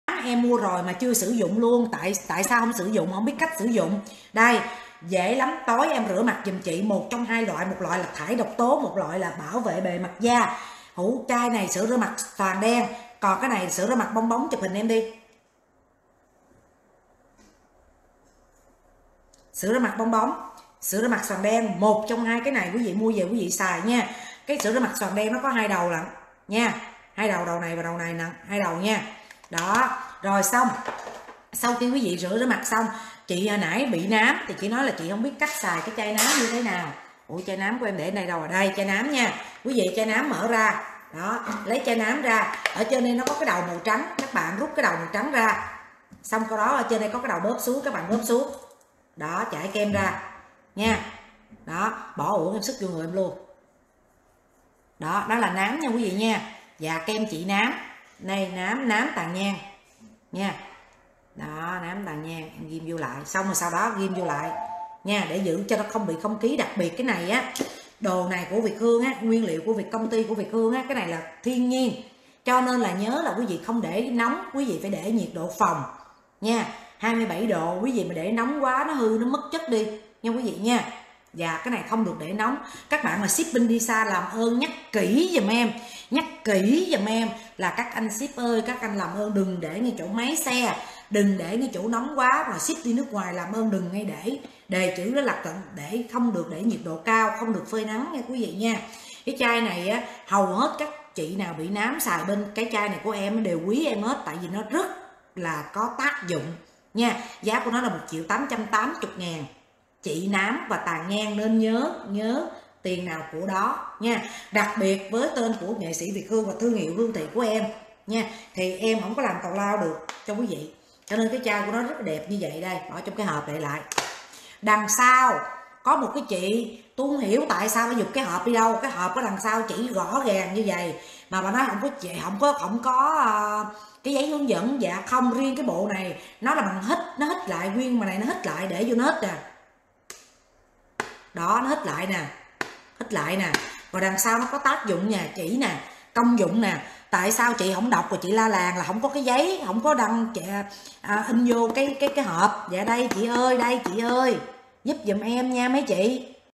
Sữa rửa mặt & Kem trị nám cao cấp
Việt Hương Livestream tại nhà riêng bên Mỹ 16/9